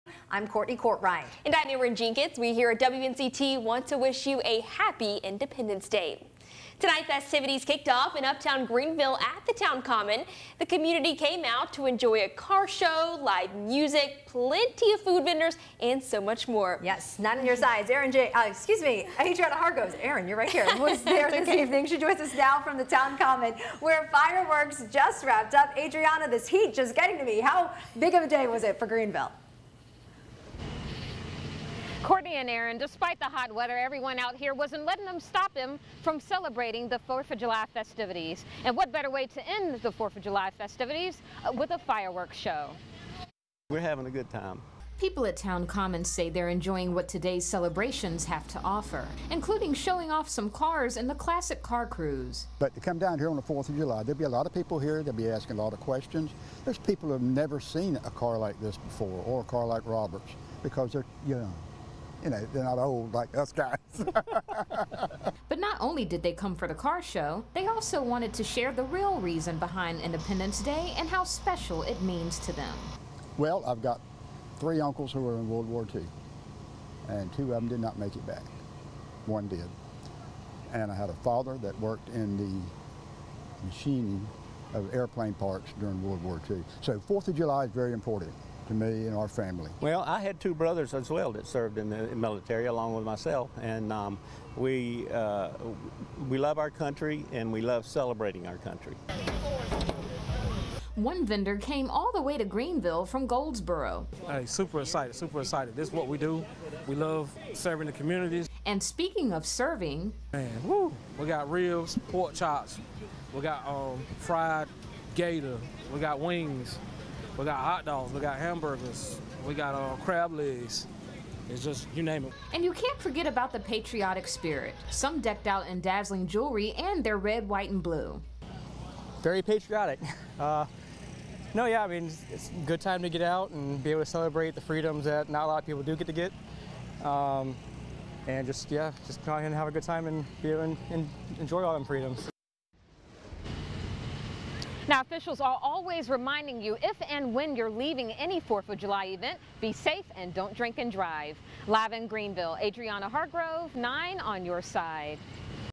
People enjoy food, music and fireworks during Fourth of July at Town Common
Greenville's Fourth of July festivities took place at Town Common on Tuesday. Adrianna Hargrove was there and spoke with several people who were taking it all in.